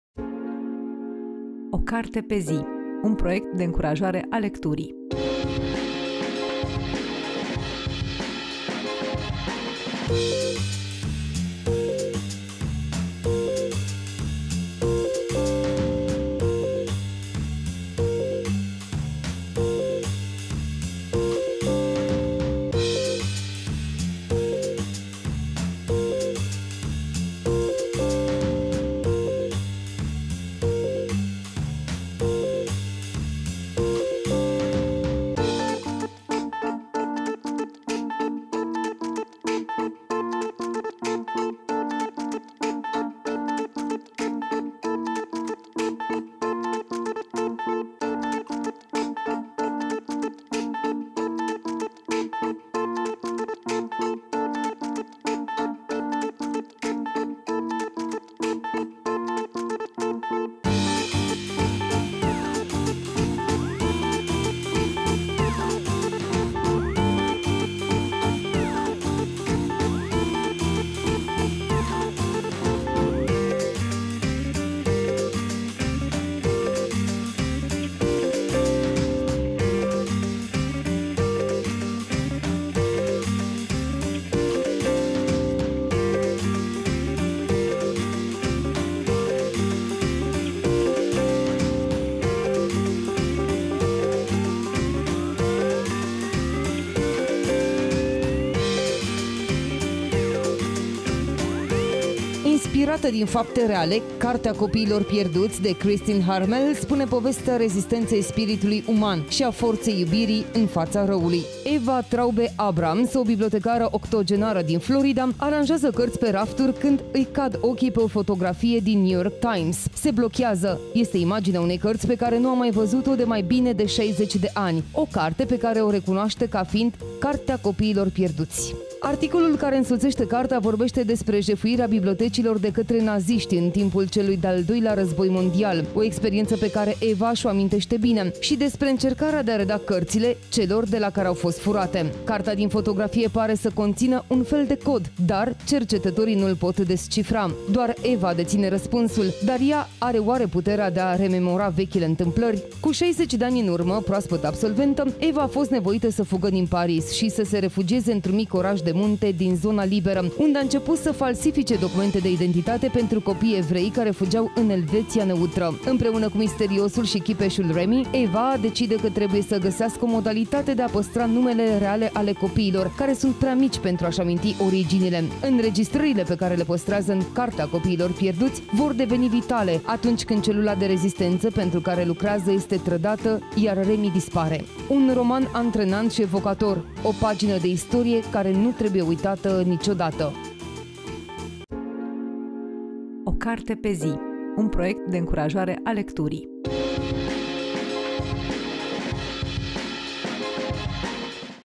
O carte pe zi: „Cartea copiilor pierduți” de Kristin Harmel
Cartea copiilor pierduți de Kristin Harmel este recomandarea zilei de joi, 23 septembrie 2021, în cadrul rubricii „O carte pe zi”.

„Harmel s-a inspirat din povestea adevărată a unor cetățeni francezi care au luptat împotriva ororilor celui de-al Doilea Război Mondial, cu curaj și convingere. (...) Tulburătoare și magnifică.” (Fiona Davis)

© un proiect Litera în parteneriat cu Digi24.